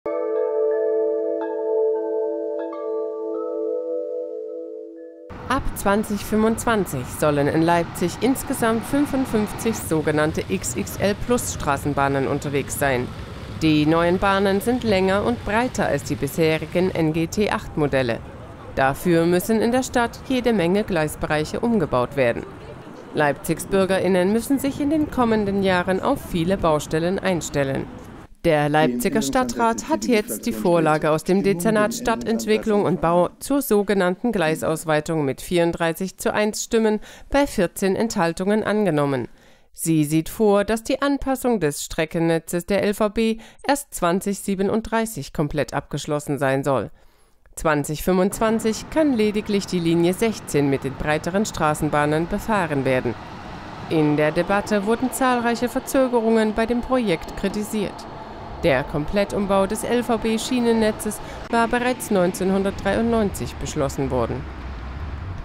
Umbau des LVB-Schienennetz | LZ TV Nachrichten
Ab 2025 sollen in Leipzig sogenannte XXL Plus-Straßenbahnen unterwegs sein, die breiter als die bisherigen Modelle sind.

Der Stadtrat genehmigt jetzt den Umbau des LVB-Schienennetz/es bis 2037.

Mehr dazu in unserem LZTV-Beitrag.